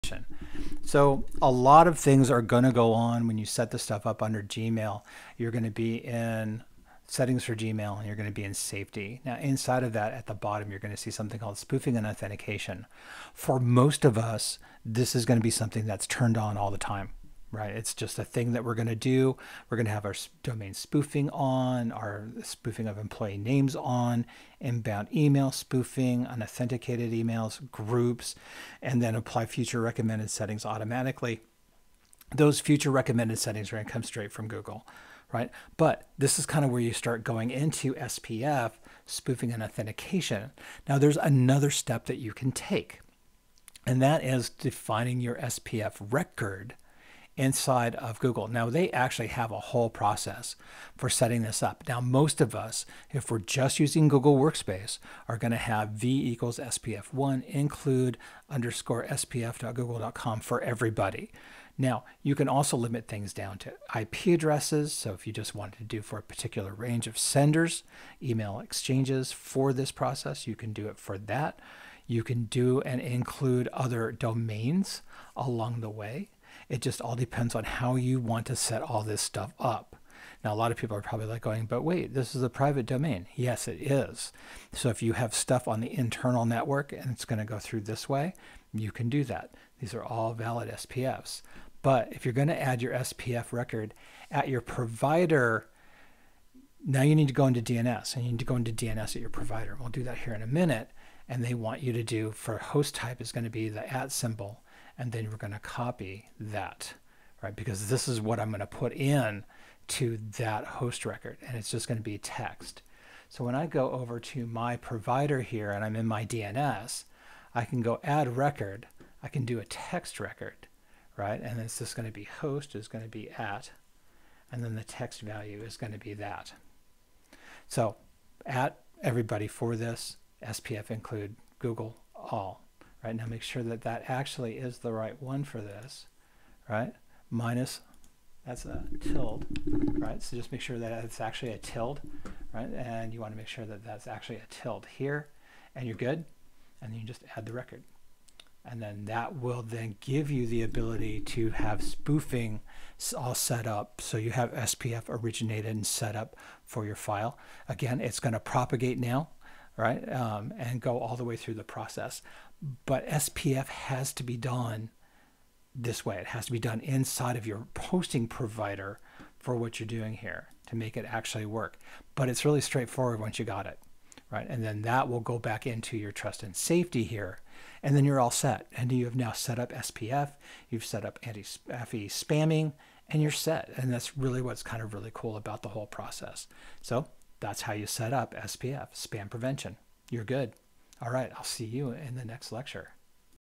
Setup SPF in Google Workspaces
This is part of our hands-on Google Workspaces Administrator course, how to set up SPF in Gmail and your own DNS system. Walks through the whole process.